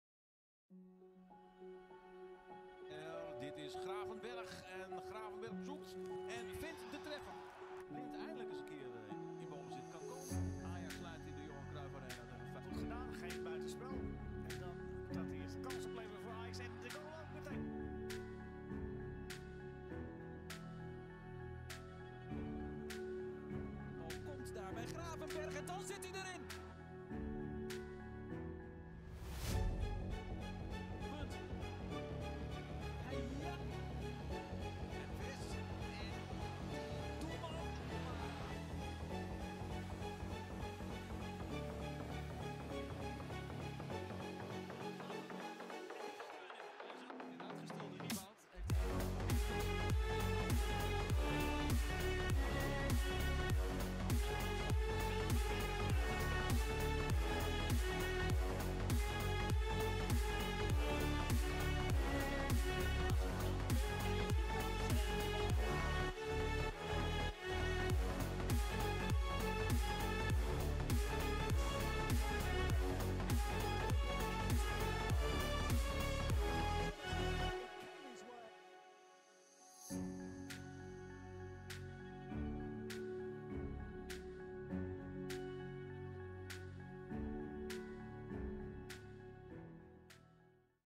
Ryan Gravenberch - All his goals in 2021 & 2022!
Liverpool player now!
Also player of the national team of the Netherlands & Netherlands U21

He came from Bayern München this summer.

How will he do in Premier League?

Watch all his goals from last two years here!

for more video's!

Thanks!

How to watch PL in your country?

Do you like playing FIFA?